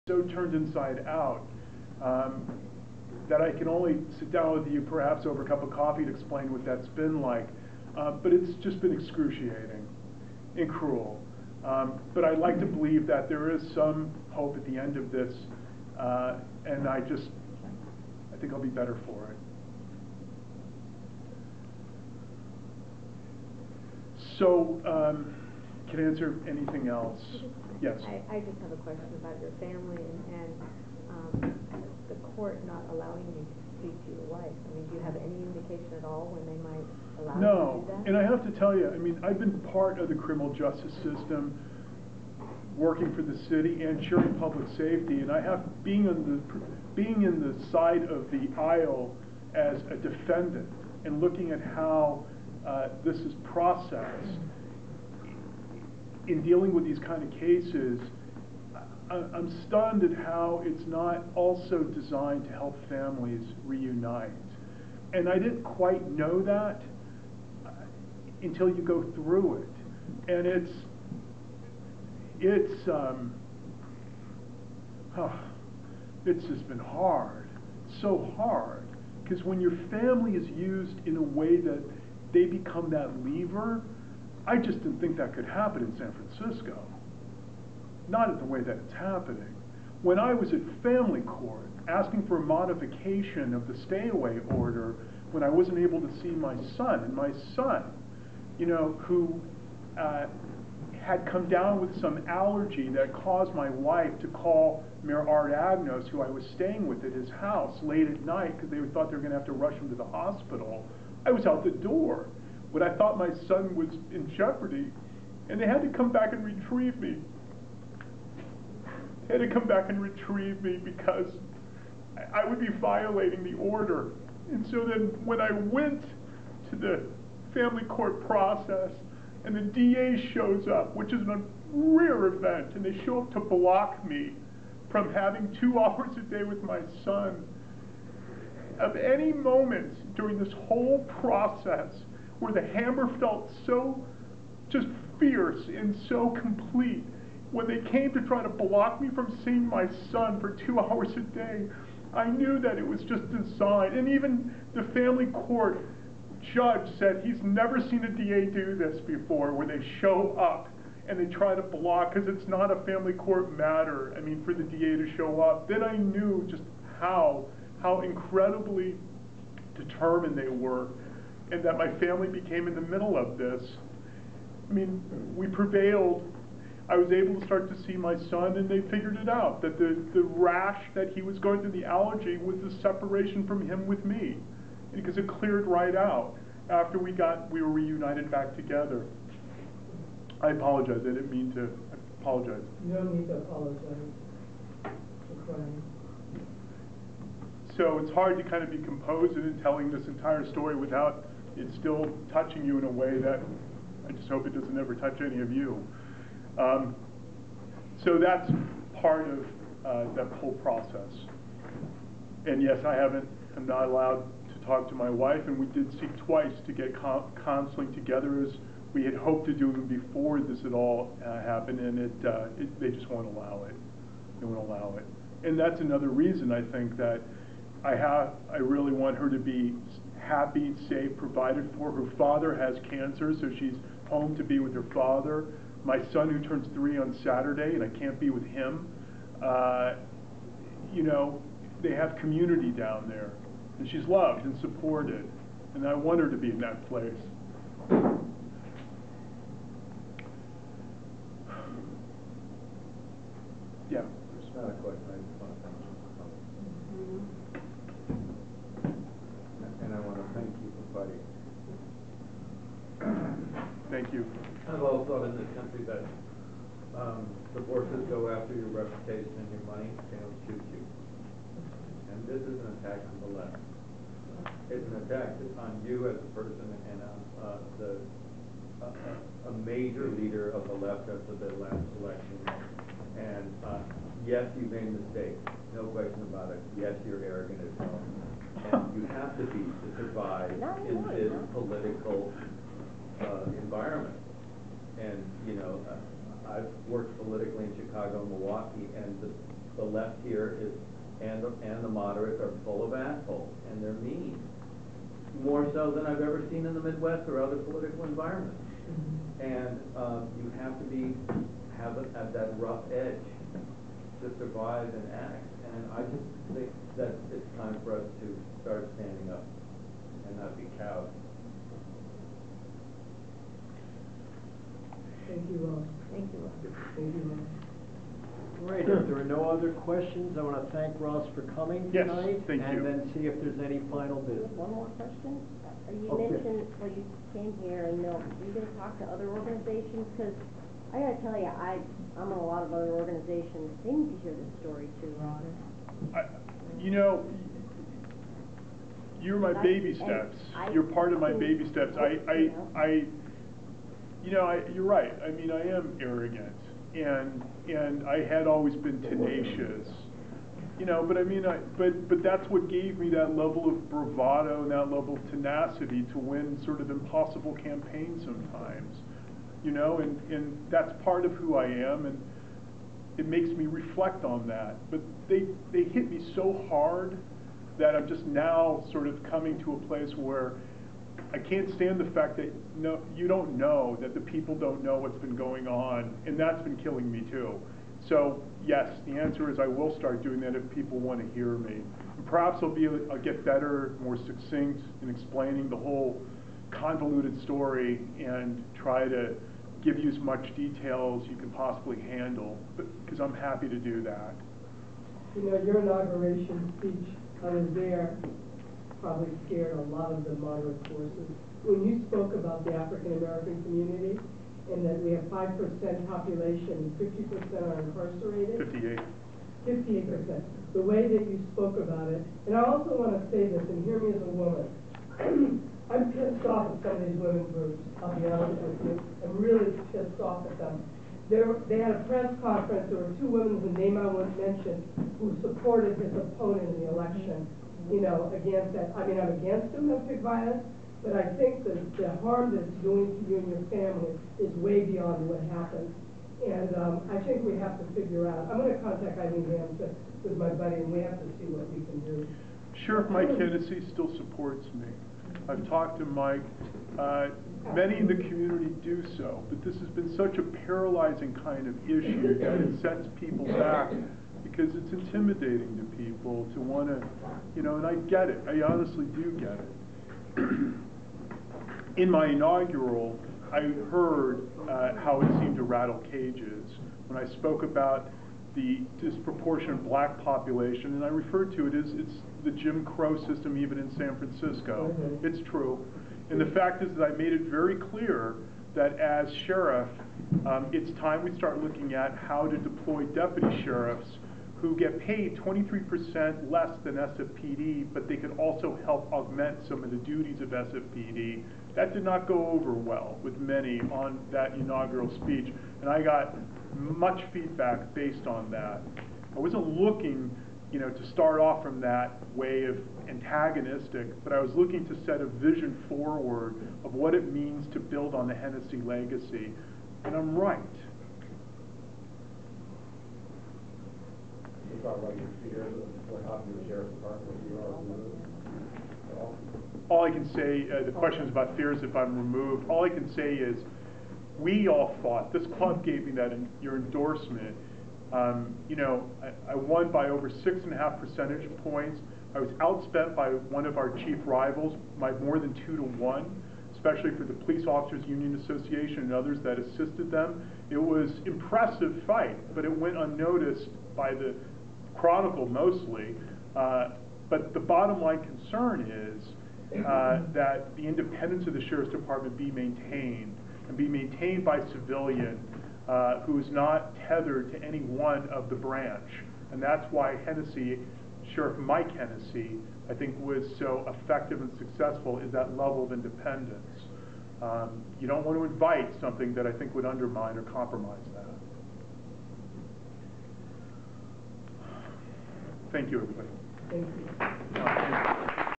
San Francisco Sheriff Ross Mirkarimi Tells His Side of the Story (3 of 3)
San Francisco Sheriff Ross Mirkarimi makes his 1st public statement about his fight to keep his job and tells his side of the incident that led to his suspension. He spoke on April 17th in front of the Bernal Heights Democratic Club.